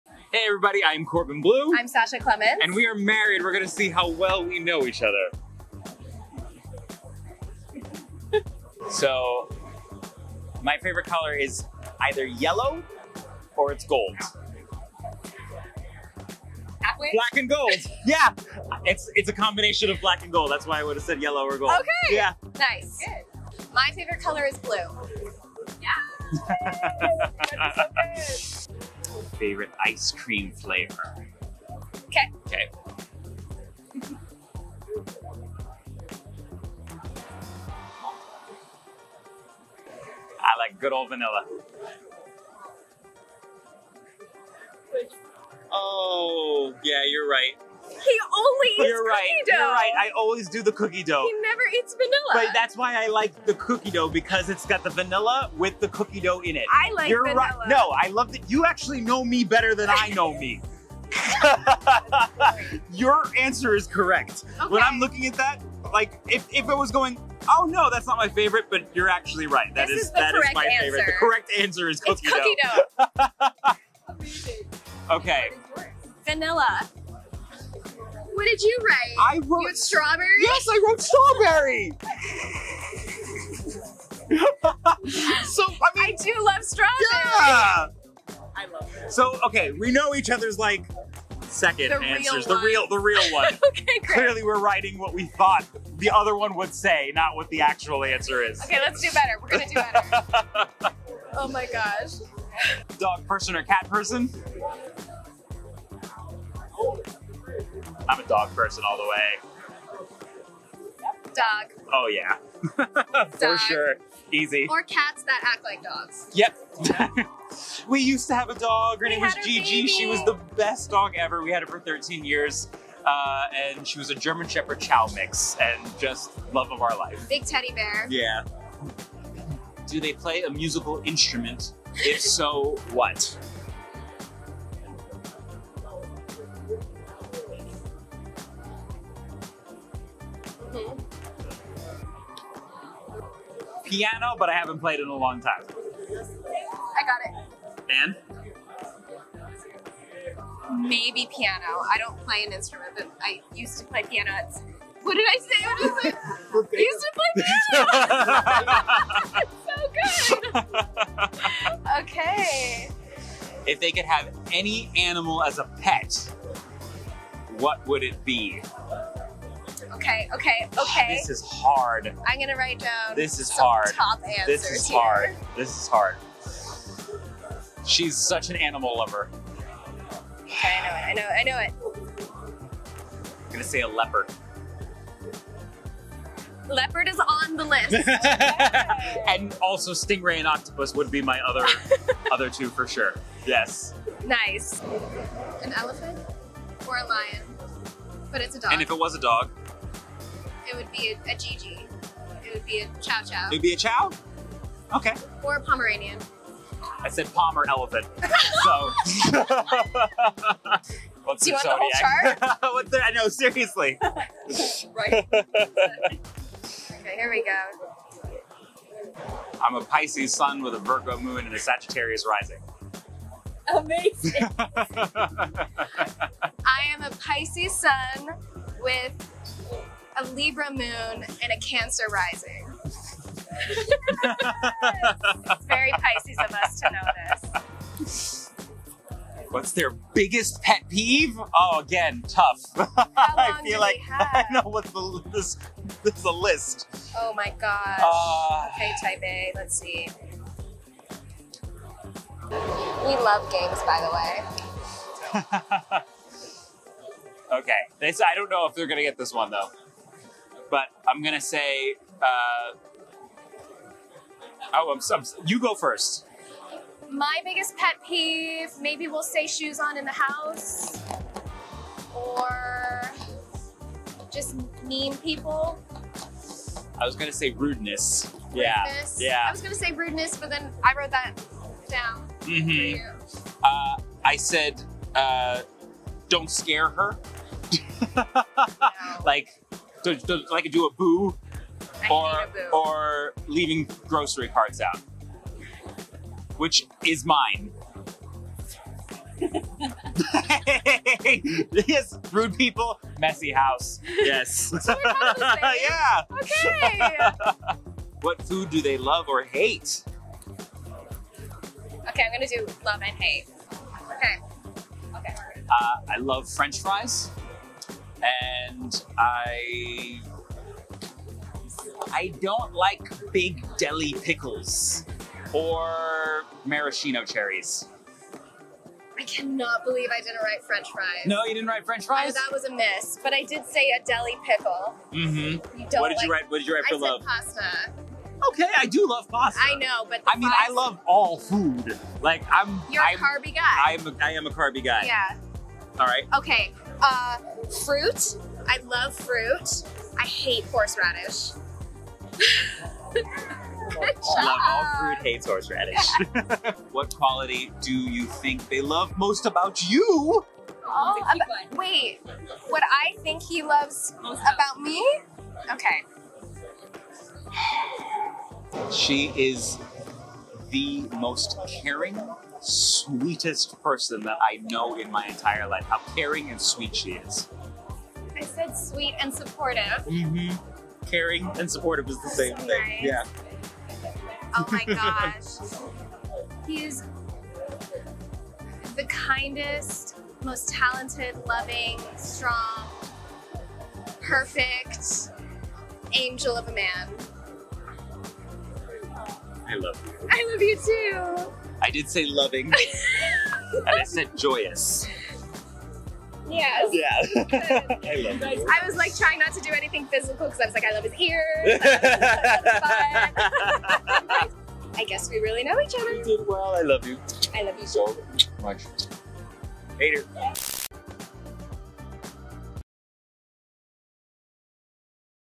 Who Knows Who With Corbin Bleu & Sasha Clements | HSM
How well do HighSchoolMusical star Corbin Bleu and his wife Sasha really know each other? Find out in today's video.